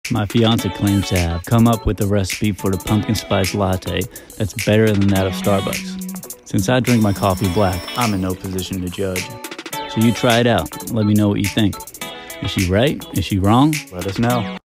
Copycat Starbucks Pumpkin Spice Latte! ☕️
Could this be BETTER than Starbucks' Pumpkin Spice Latte?!
Recipe 👀:
1 TBSP Pumpkin Puree.
2 TBSP Heavy Whipping Cream.
1 Cup of Milk (of your choice).
* Add Cinnamon and Sugar to your liking*